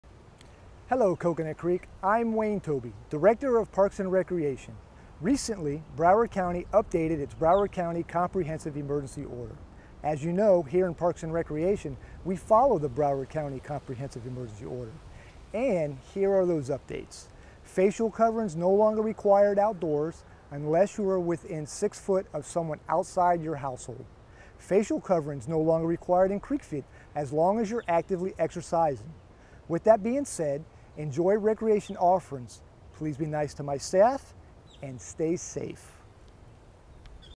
Parks and Recreation to follow the Broward County Comprehensive Emergency Order
Parks and Recreation Director Wayne Tobey